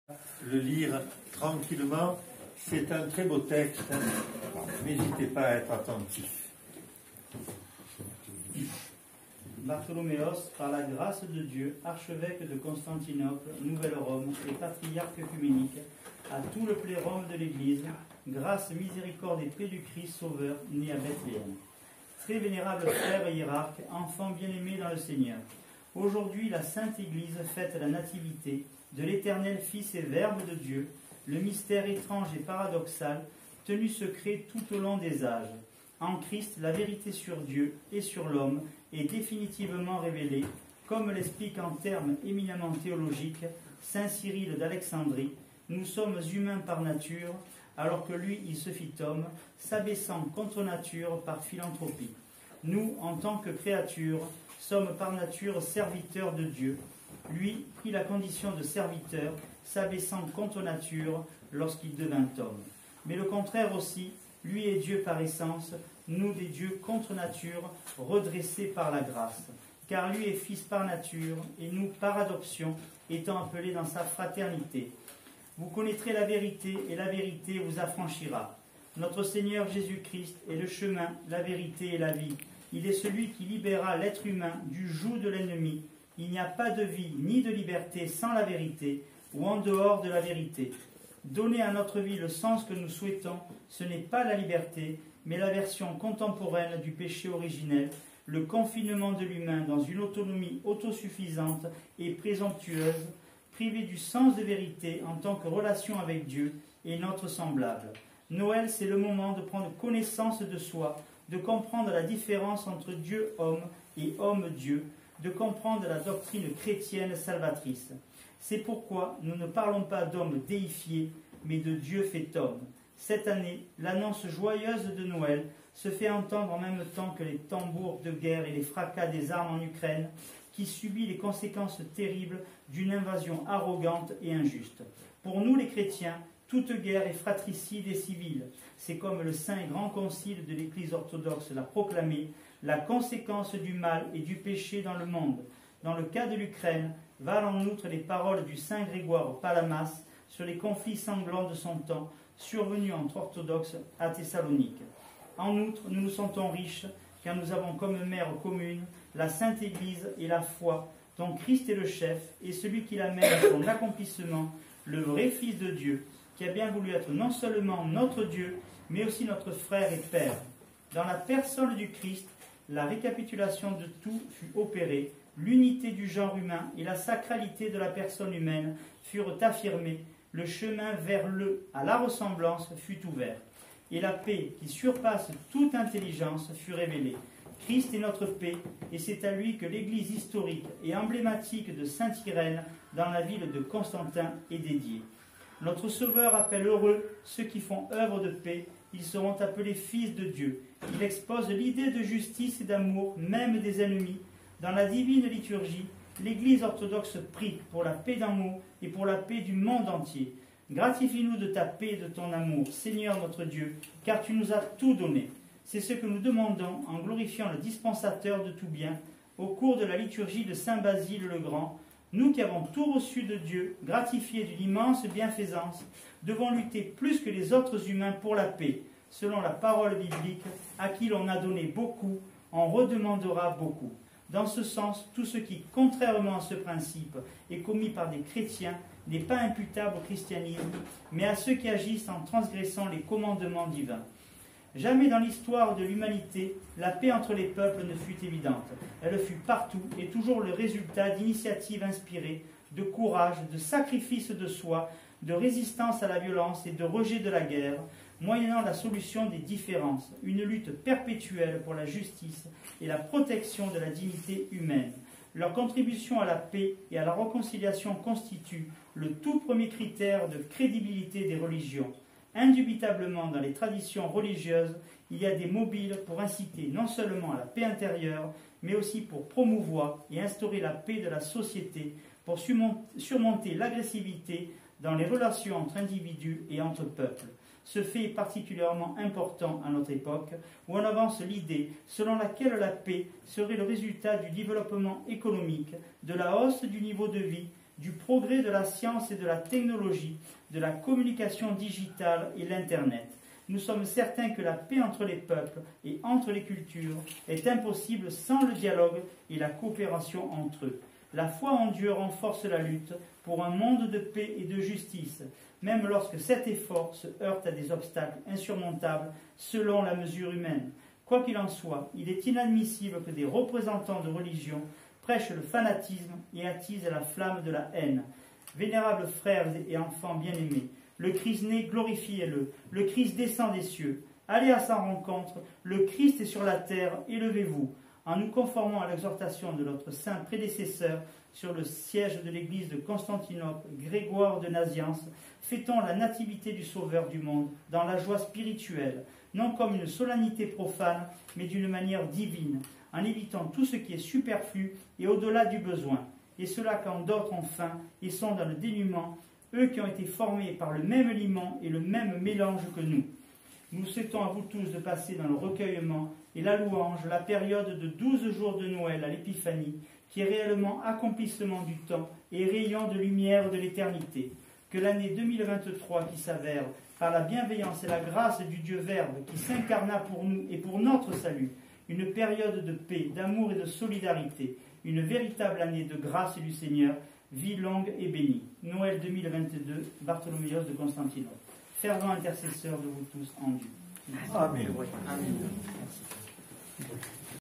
Fête de la Nativité de notre seigneur 24 décembre 2022
message lu par le lecteur Jérôme lors de la fête de la Nativité de notre seigneur . Église orthodoxe grecque Saint Cosme et Saint Damien Avignon